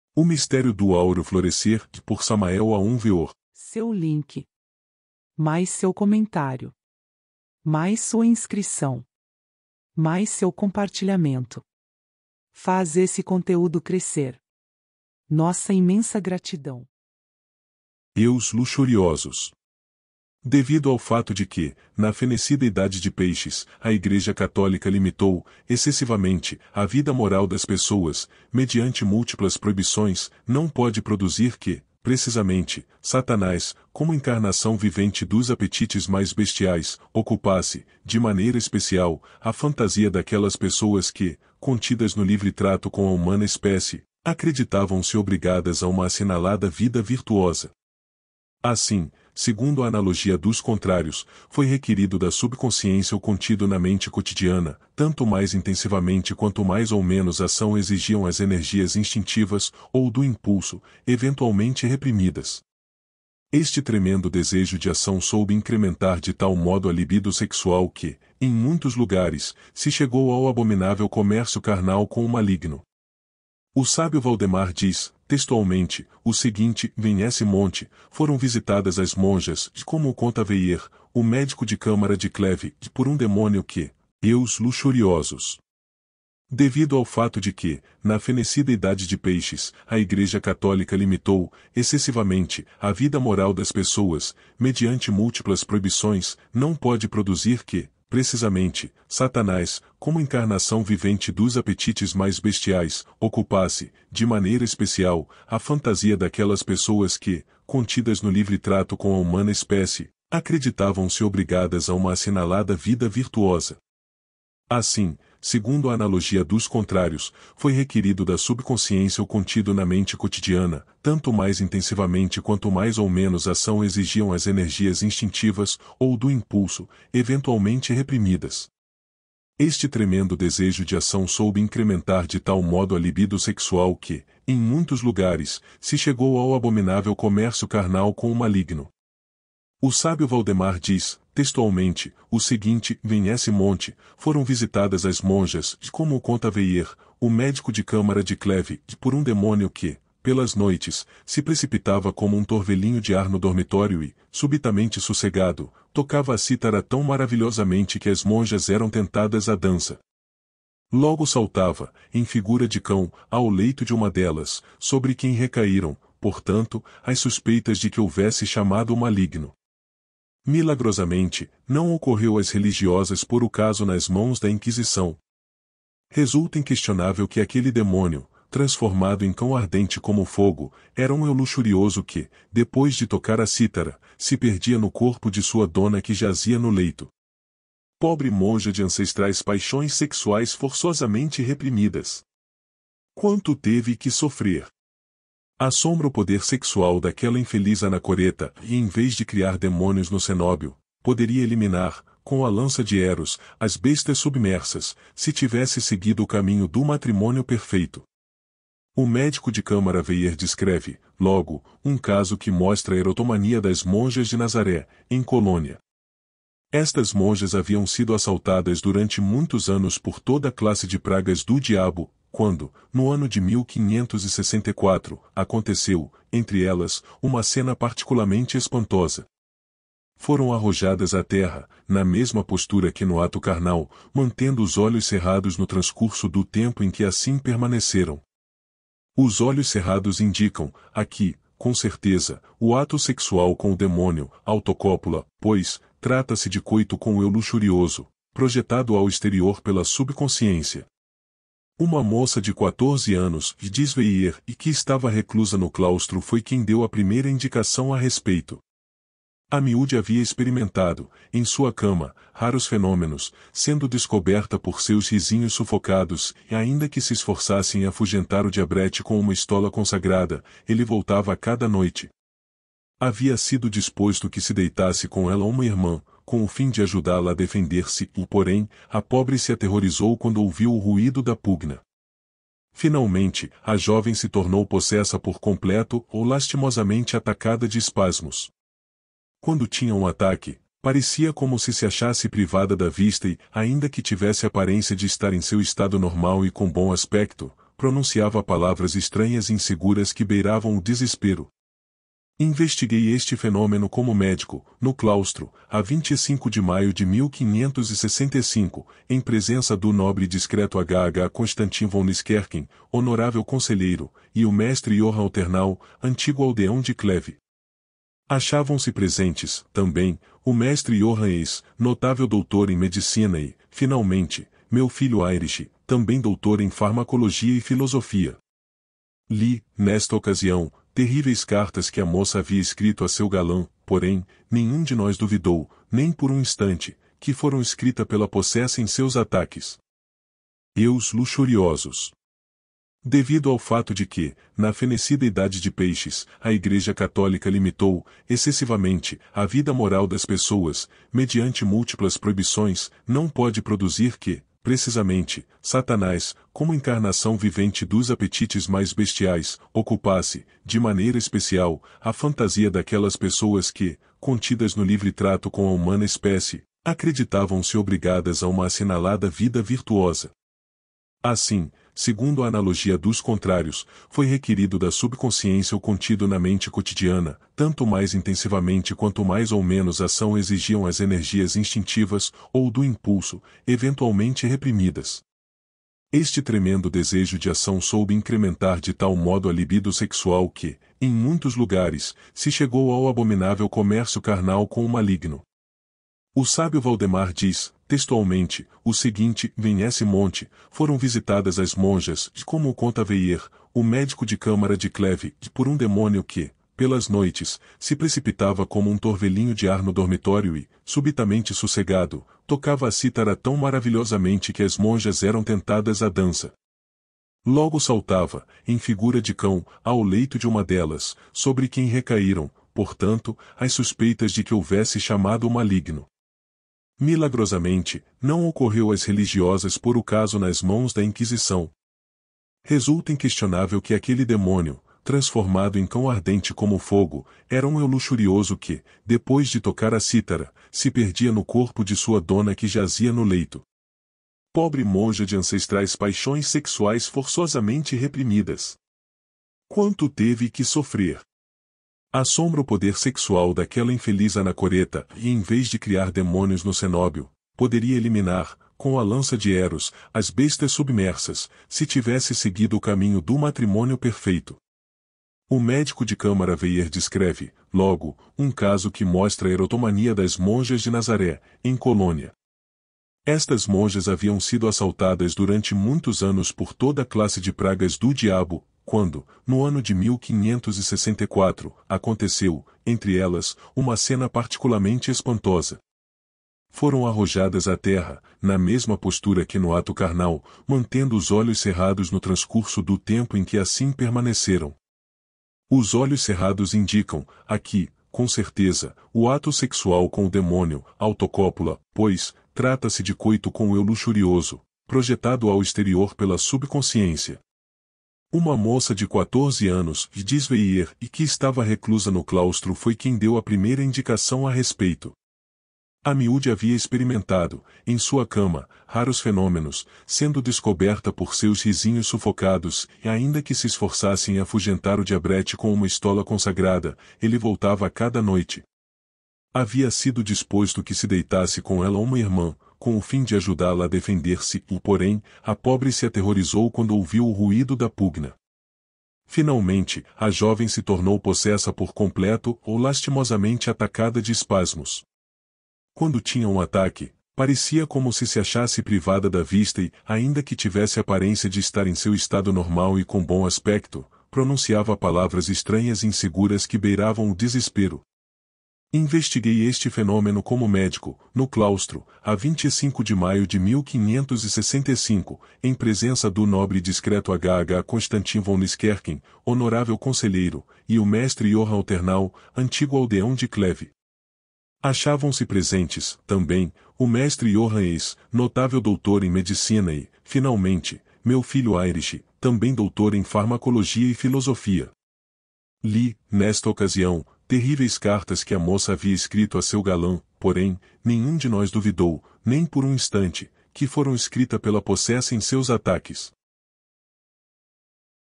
Eus Luxuriosos Capítulo VII O Mistério do Áureo Florescer – por Samael Aun Weor
O Mistério do Áureo Florescer
Samael Aun Weor
Eus Luxuriosos
Capítulo VII
Devido ao fato de que, na fenecida Idade de Peixes, a Igreja Católica limitou, excessivamente, a vida moral das pessoas, mediante múltiplas proibições, não pode produzir que, precisamente, Satanás, como encarnação vivente dos apetites mais bestiais, ocupasse, de maneira especial, a fantasia daquelas pessoas que, contidas no livre trato com a humana espécie, acreditavam−se obrigadas a uma assinalada vida virtuosa.

Assim, segundo a analogia dos contrários, foi requerido da subconsciência o contido na mente cotidiana, tanto mais intensivamente quanto mais ou menos ação exigiam as energias instintivas, ou do impulso, eventualmente reprimidas.

Este tremendo desejo de ação soube incrementar de tal modo a libido sexual que, em muitos lugares, se chegou ao abominável comércio carnal com o maligno.

O sábio Waldemar diz, textualmente, o seguinte: “Em Hessimont, foram visitadas as monjas – como o conta Wyer, o médico de câmara de Clewe – por um demônio que, pelas noites, se precipitava como um torvelinho de ar no dormitório e, subitamente sossegado, tocava a cítara tão maravilhosamente que as monjas eram tentadas à dança.”

“Logo saltava, em figura de cão, ao leito de uma delas, sobre quem recaíram, portanto, as suspeitas de que houvesse chamado o maligno.” Milagrosamente, não ocorreu às religiosas pôr o caso nas mãos da Inquisição.

Resulta inquestionável que aquele demônio, transformado em cão ardente como o fogo, era um eu luxurioso que, depois de tocar a cítara, se perdia no corpo de sua dona que jazia no leito.

Pobre monja de ancestrais paixões sexuais forçosamente reprimidas! Quanto teve que sofrer!

Assombra o poder sexual daquela infeliz anacoreta; em vez de criar demônios no cenóbio, poderia eliminar, com a lança de Eros, as bestas submersas, se tivesse seguido o Caminho do Matrimônio Perfeito.

O médico de câmara Wyer descreve, logo, um caso que mostra a erotomania das monjas de Nazaré, em Colônia.

“Estas monjas haviam sido assaltadas durante muitos anos por toda classe de pragas do diabo; quando, no ano de 1564, aconteceu, entre elas, uma cena particulamente espantosa. Foram arrojadas à terra, na mesma postura que no ato carnal, mantendo os olhos cerrados no transcurso do tempo em que assim permaneceram.” Os olhos cerrados indicam, aqui, com certeza, o ato sexual com o demônio, autocópula; pois, trata−se de coito com o eu luxurioso, projetado ao exterior pela subconsciência.
“Uma moça de quatorze anos – diz Wyer – que estava reclusa no claustro foi quem deu a primeira indicação a respeito.”

“Amiúde havia experimentado, em sua cama, raros fenômenos, sendo descoberta por seus risinhos sufocados e, ainda que se esforçasse em afugentar o diabrete com uma estola consagrada, ele voltava a cada noite.”

“Havia sido disposto que se deitasse com ela uma irmã, com o fim de ajudá−la a defender−se; porém, a pobre se aterrorizou quando ouviu o ruído da pugna.”

“Finalmente, a jovem se tornou possessa por completo; lastimosamente atacada de espasmos.”
“Quando tinha um ataque, parecia como se se achasse privada da vista e, ainda que tivesse aparência de estar em seu estado normal e com bom aspecto, pronunciava palavras estranhas e inseguras que beiravam o desespero.”

“Investiguei este fenômeno como médico, no claustro, a 25 de maio de 1565, em presença do nobre e discreto H.H. Constantin Von Lyskerken, honorável conselheiro, e o Mestre Johann Alternau, antigo aldeão de Clewe.”